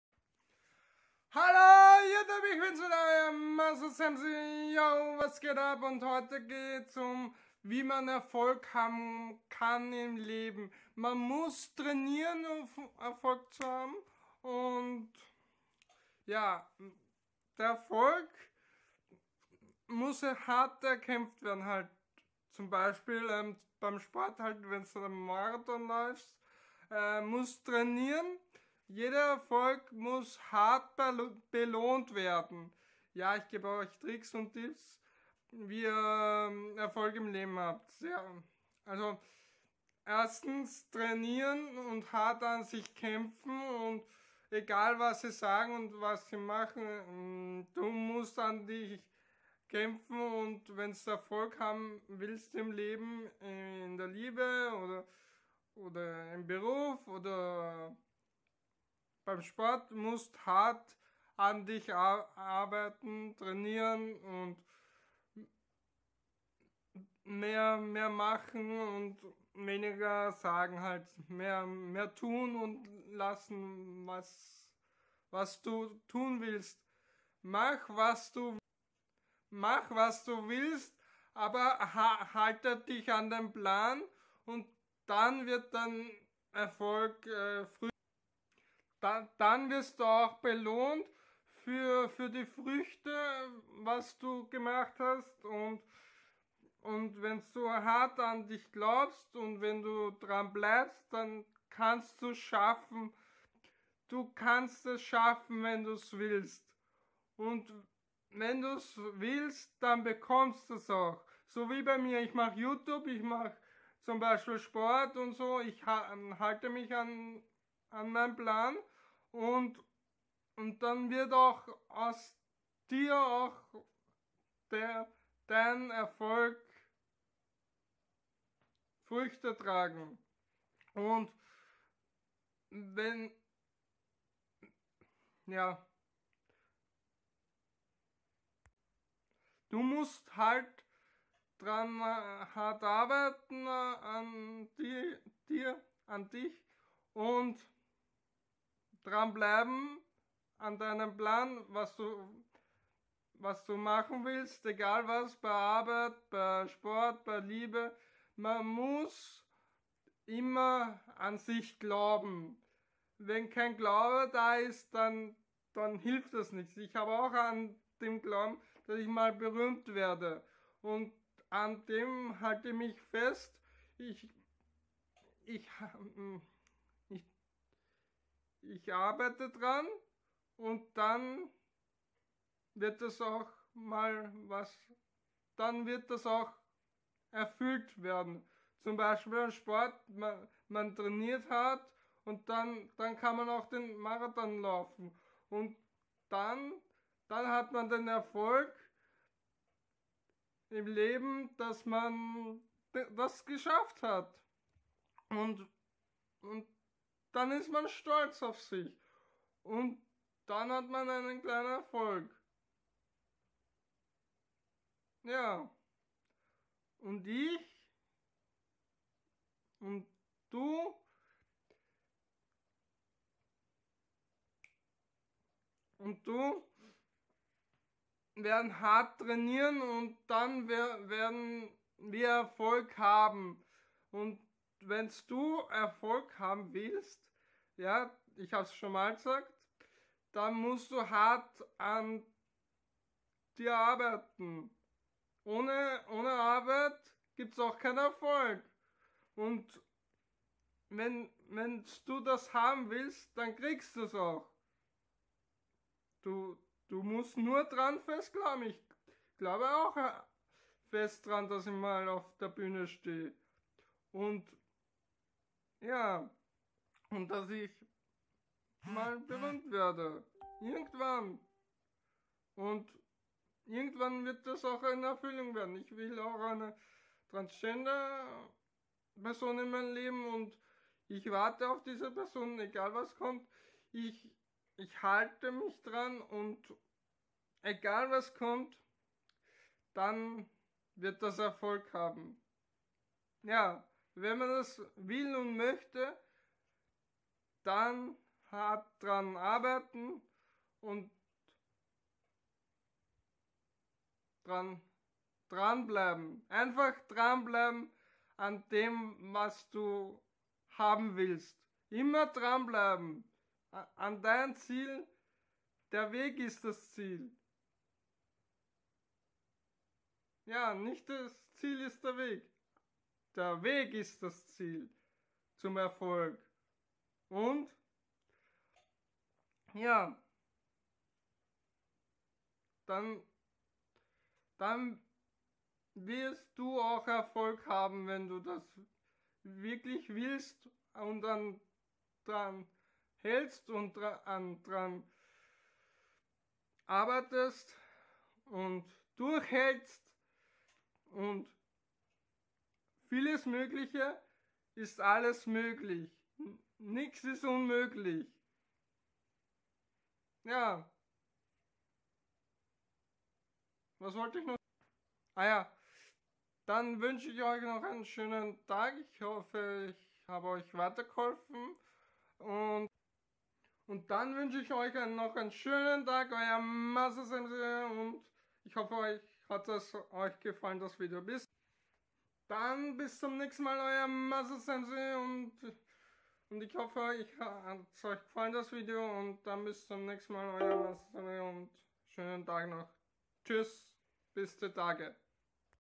Erfolg haben im leben
Hey leute
Heute in diesem video geht es um wie ihr erfolg haben kannst und du auch deine ziele erfüllst und wie Du dort hin bekommst und erfolg auch haben in der liebe, arbeit oder sport und vieles mehr,dass beantworte ich dir in diesem video. Viel spaß mit diesem video wünscht euch euer masterzemsi